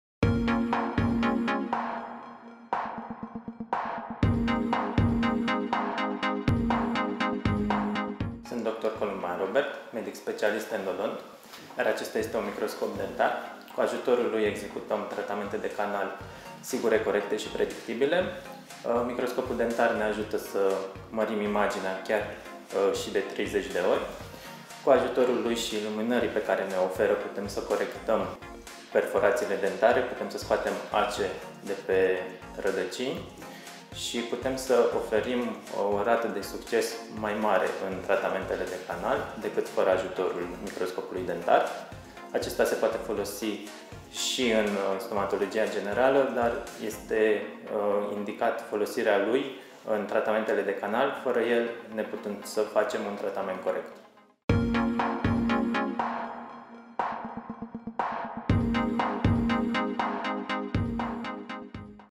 Endodonția la microscop: succes și profesionalism. Dr. Robert Kolumban - Clinica EXCEDENT
Microscopul endodontic aduce un beneficiu major in salvarea dintilor proprii afectati de evolutia cariilor dentare, fracturilor sau perforatiilor radiculare.
Dr. Robert Kolumban - medicul specialist endodont al Clinicii EXCEDENT are o experienta vasta de lucru sub microscop atat in tratamente endodontice, cat si in extractia corpurilor straine din canale ( ace rupte, pivoti de fibra de sticla) sau din implanturile dentare ( suruburi rupte, depuneri exogene etc.
Rapid, eficient si delicat, endodontia la microscop salveaza dinti la Clinica EXCEDENT !